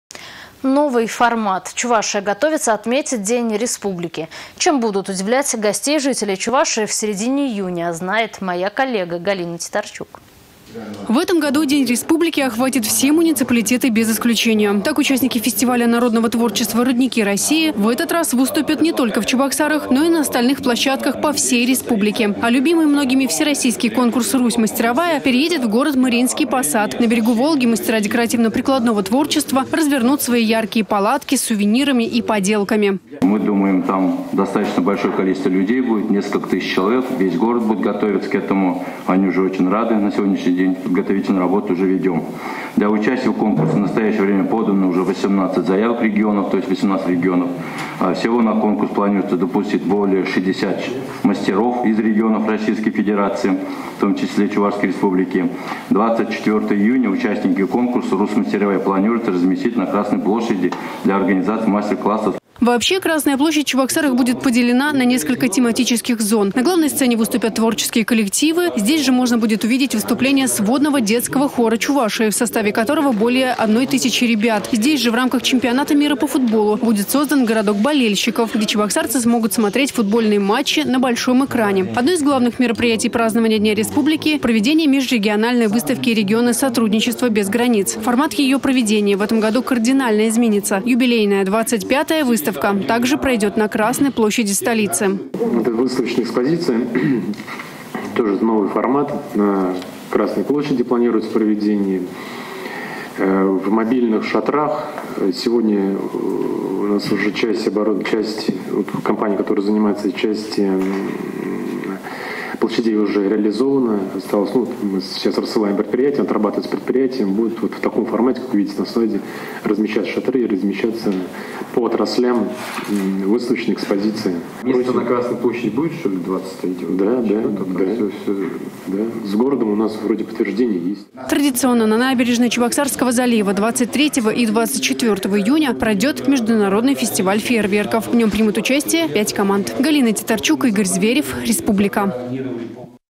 Чувашия готовится отметить День республики.
Чем будут удивлять гостей и жителей Чувашии в середине июня, знает моя коллега Галина Титарчук.